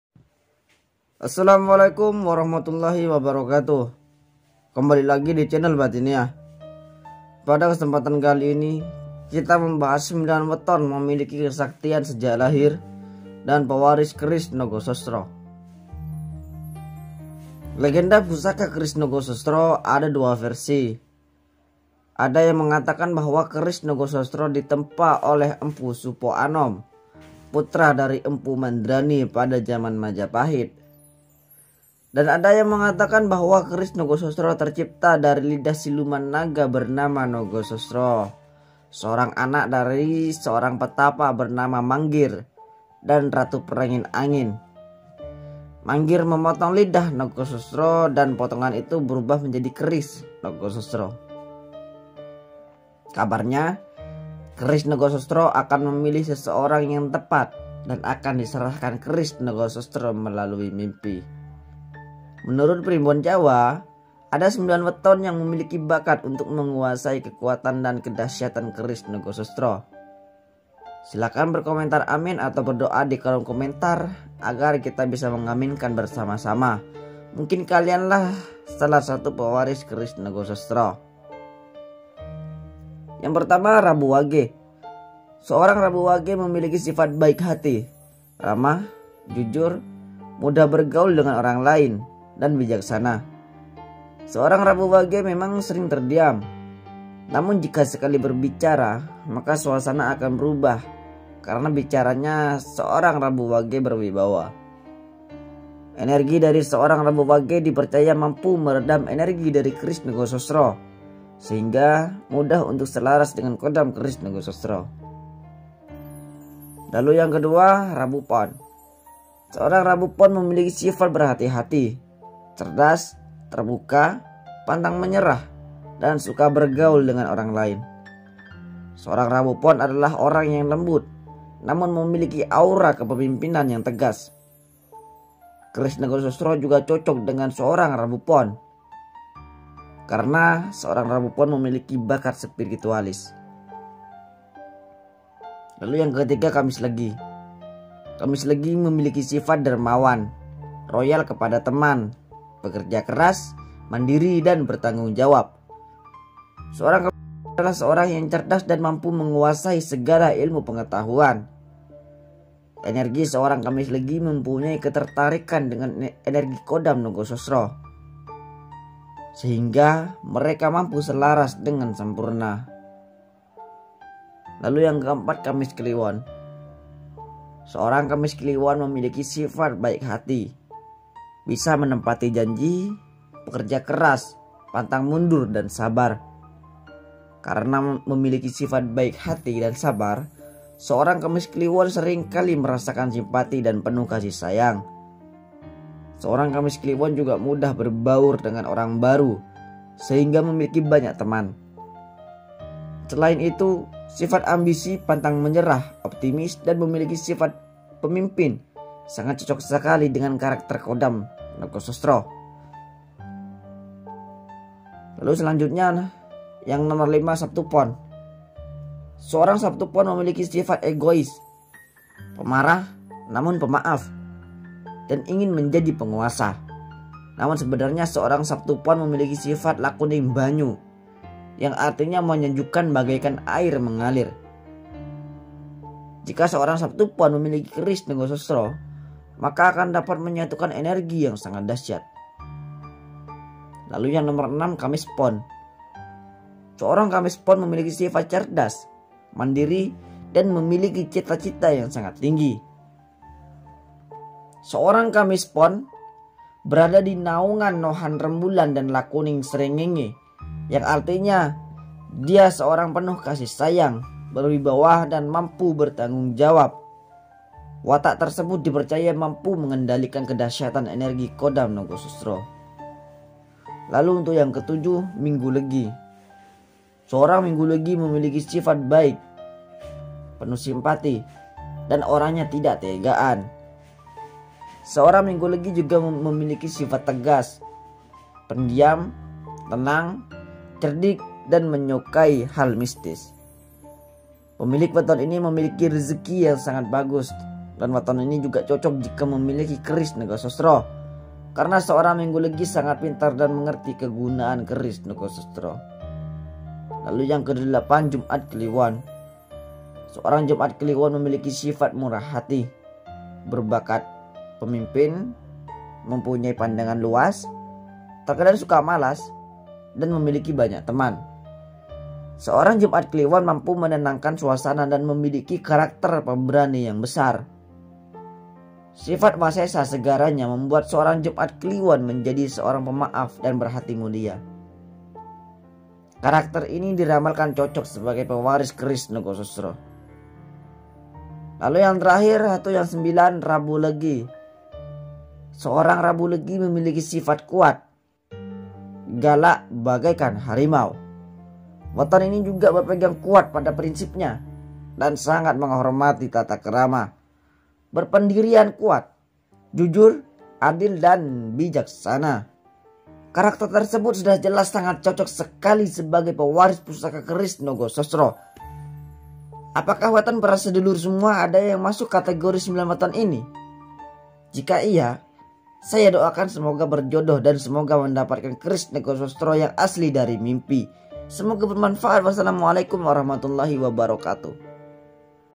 9 weton pewaris khodam dan pusaka keris nogo sosro || primbon jawa || batiniyah
9 weton pewaris khodam dan pusaka keris nogo sosro || primbon jawa || batiniyah
pada kesempatan kali ini kita akan membahas 9 weton kelahiran yang di ramal sebagai weton pewaris khodam dan pusaka keris nogo sosro